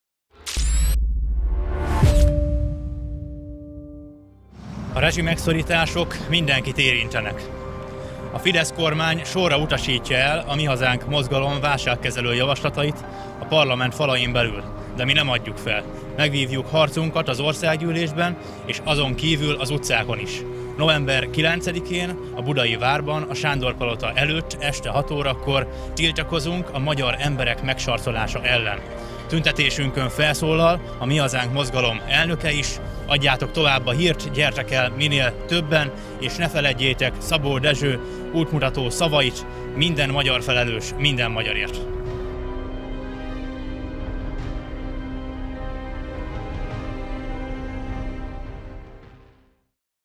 Várjuk Önöket november 9-én a Szent György téren!
November 9-én fáklyás tüntetés lesz a Szent György téren, a Mi Hazánk Mozgalom számít a jelenlétedre!